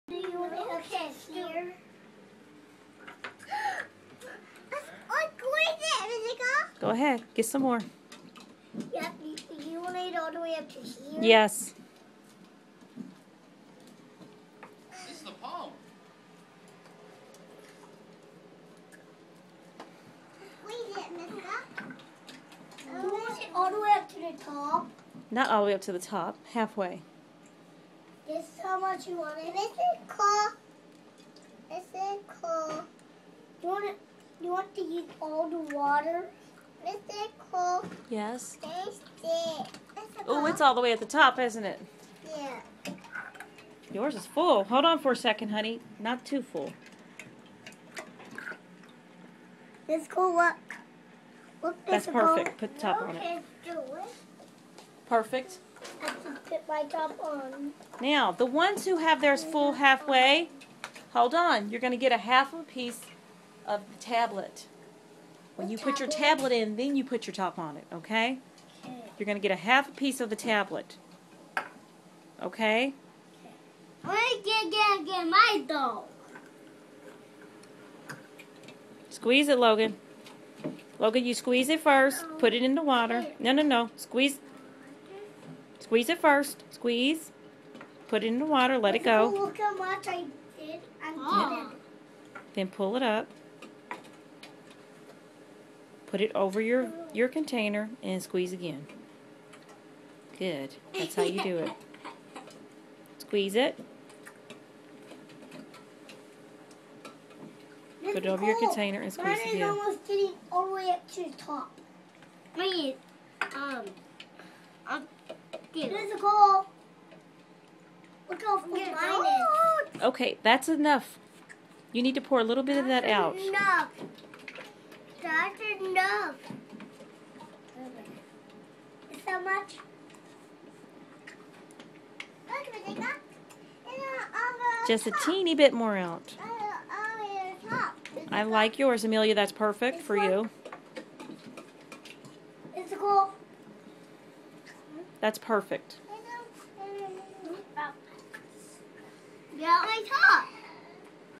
Filling Up with Pipettes
The kids were asked to fill up their film canisters half way full with water using Pipettes.